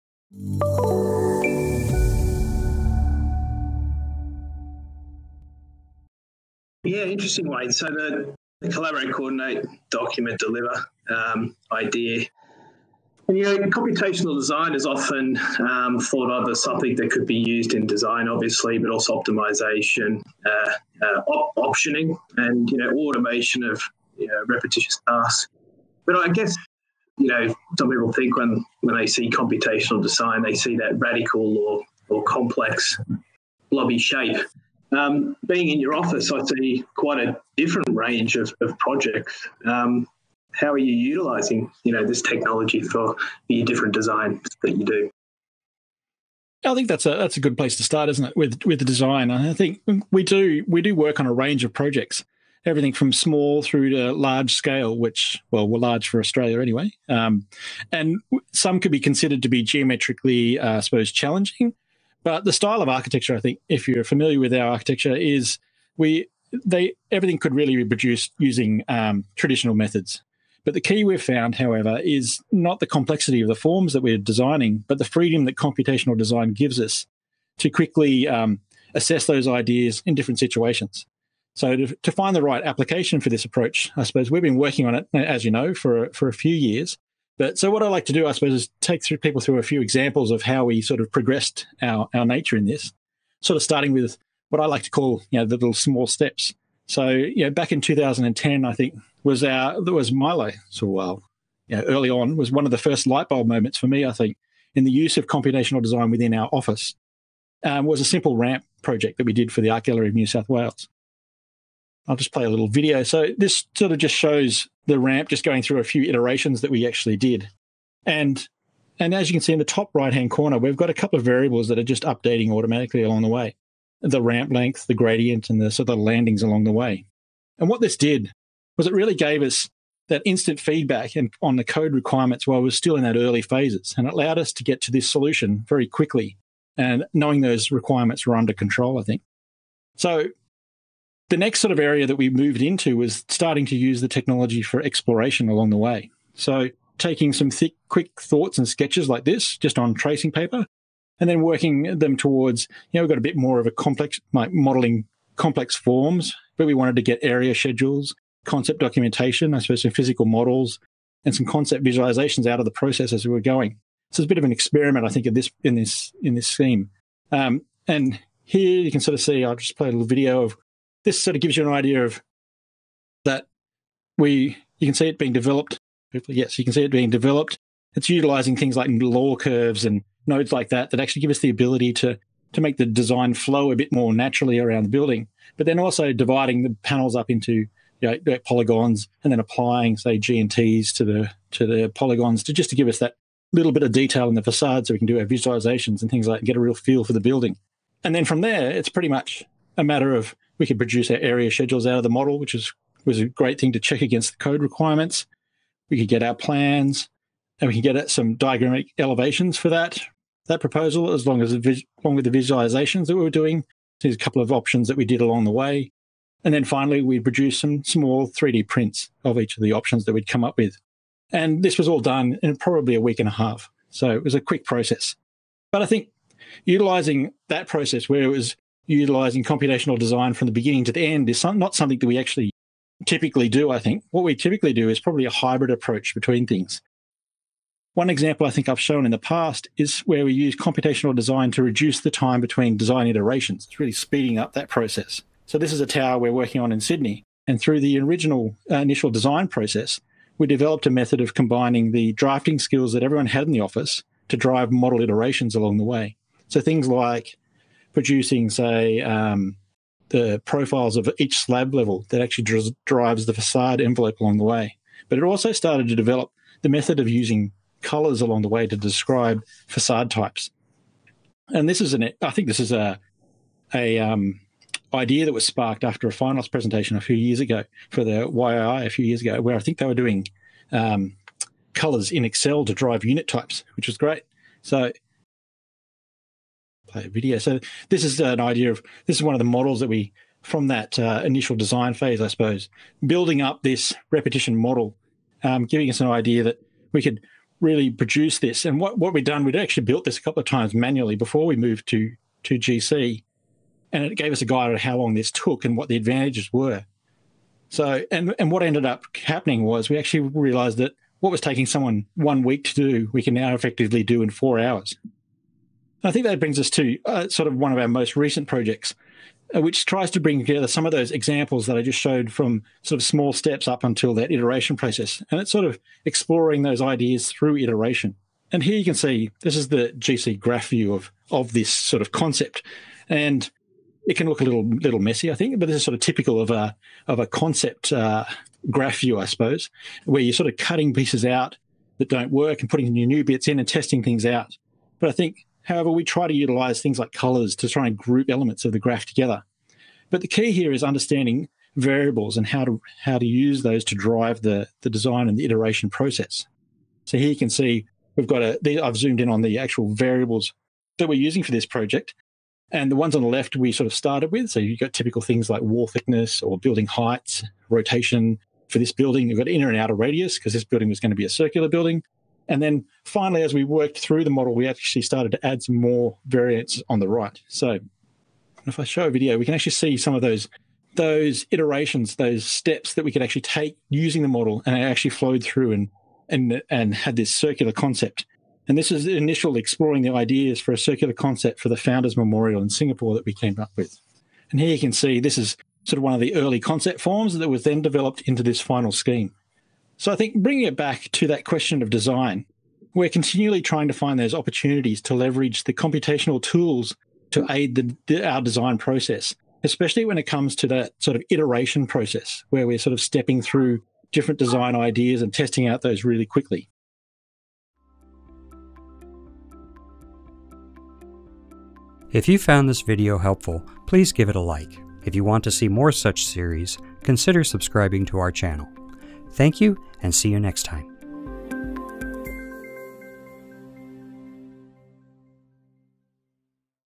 Exploration and Iterations in Design
How does computational design fit into the design process?  Wayne Dickerson will show several examples where JPW has used GenerativeComponents during the design phase to explore various design options and create multiple iterations of design solutions in far less time than other traditional design methods.